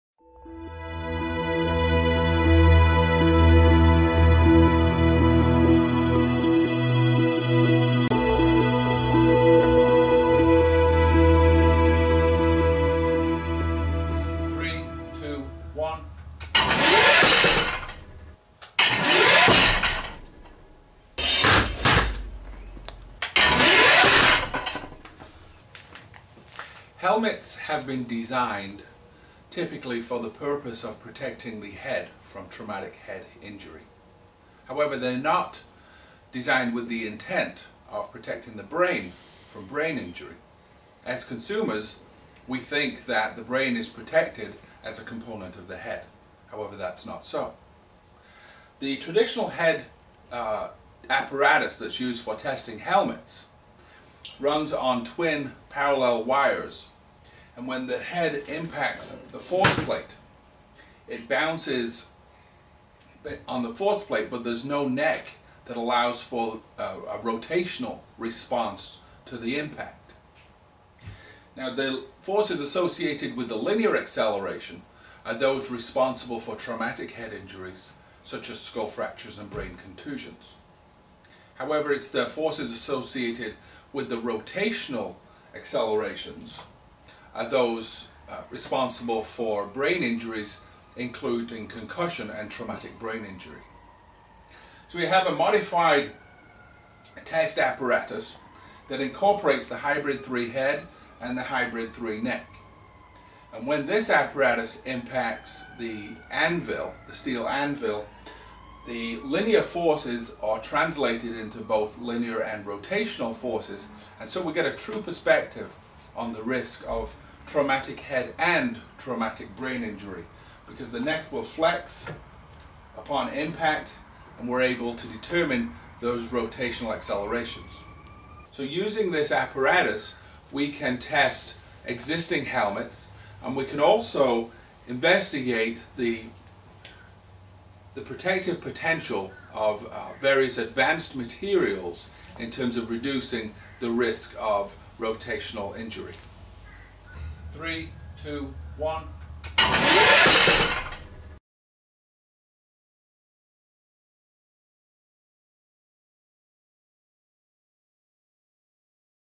Biomechanics of Football Helmets
Dr. John Lloyd discusses testing of football helmets and how typical impact testing does not measure concussion risk. A new test is demonstrated which measures the risk of brain injury. This test has been used to rank football helmets in terms of their ability to protect against both traumatic head injuries and traumatic brain injury.